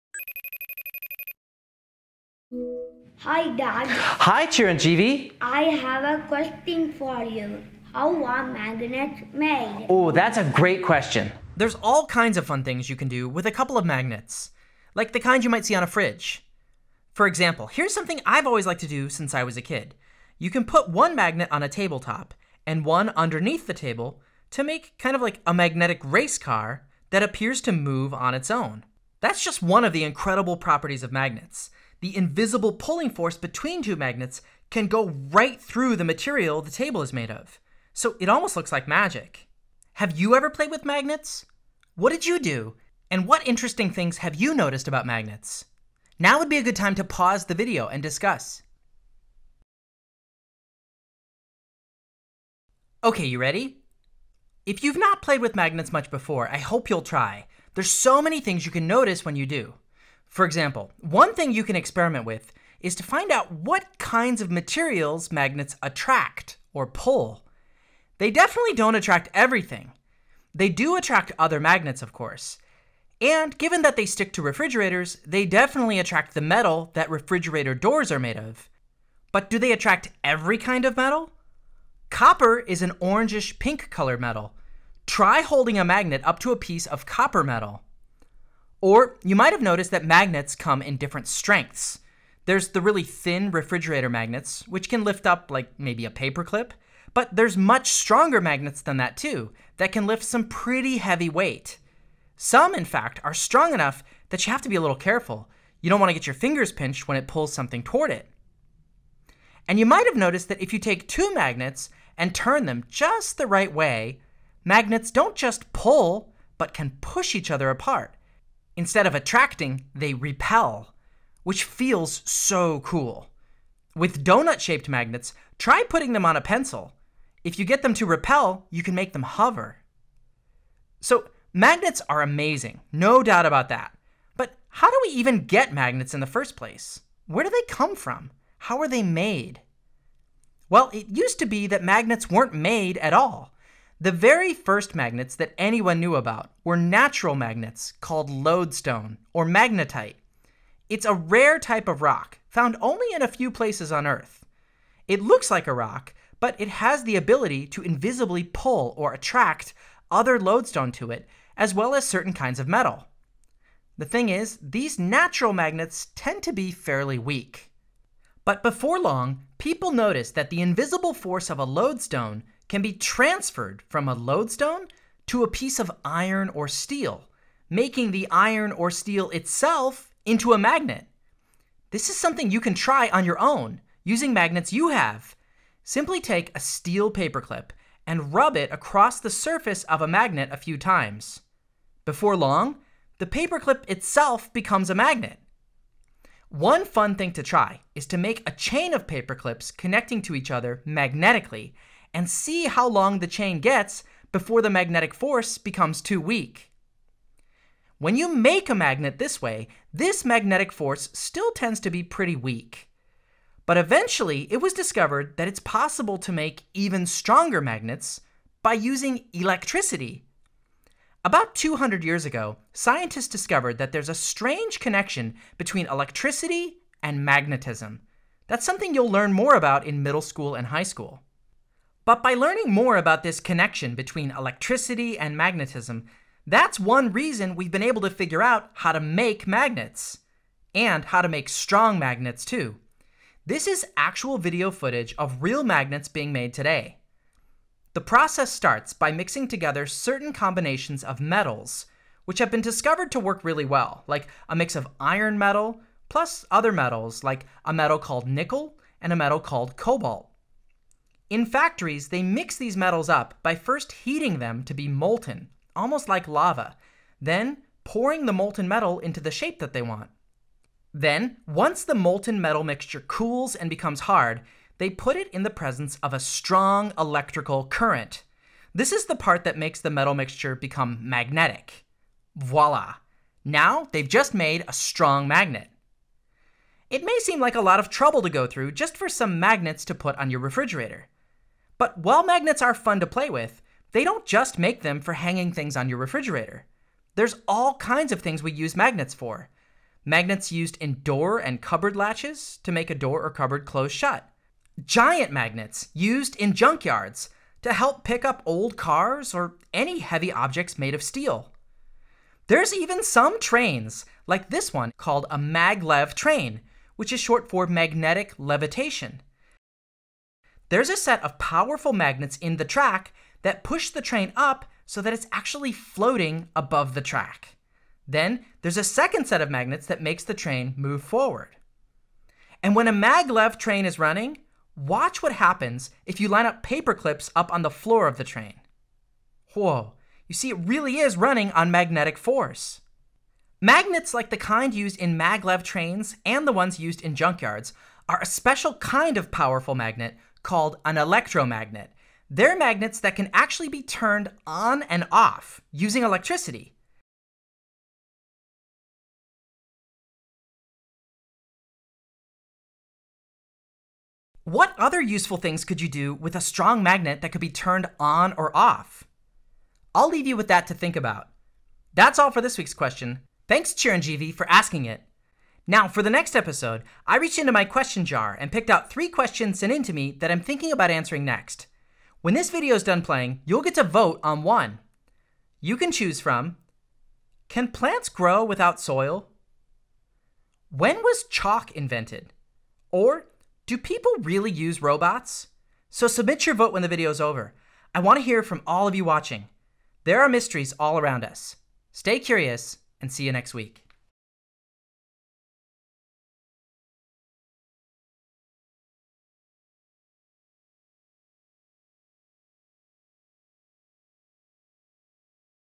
How are magnets made?
Are your kids wondering: How are magnets made? This question came from Chiranjeve, a student from the United States.